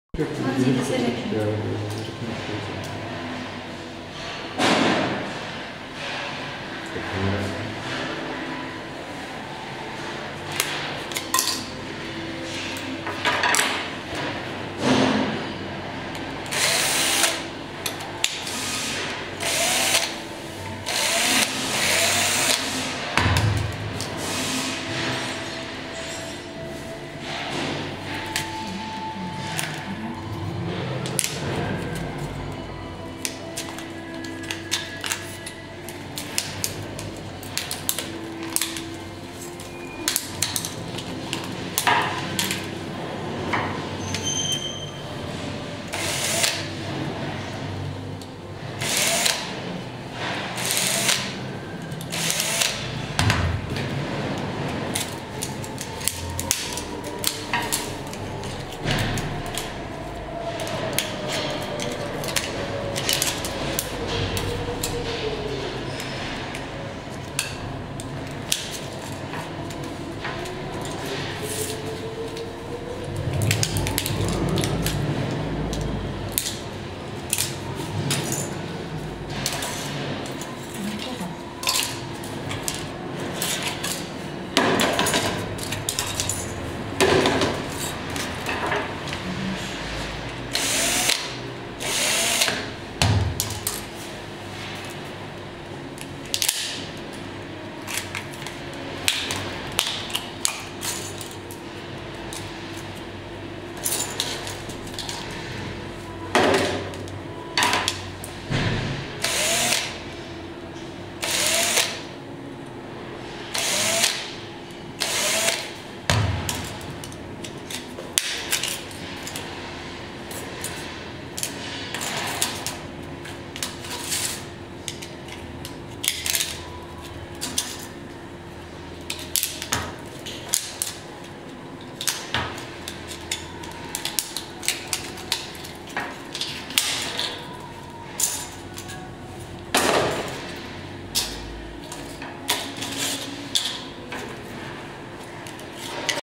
PROJEKTY Z PRAXE: Recyklace mobilů
Návštěva recyklačního zařízení na elektroodpady v Trutnově. Detail ruční demontáže mobilních telefonů, které pomáhají k následné recyklaci. Rozdělení základních částí mobilního telefonu probíhá ručně z důvodu snahy o roztřídění surovin v co nejvyšší kvalitě. U nových chytrých telefonů je demontáž & recyklace novou výzvou. Demontáž je často ručně nemožná.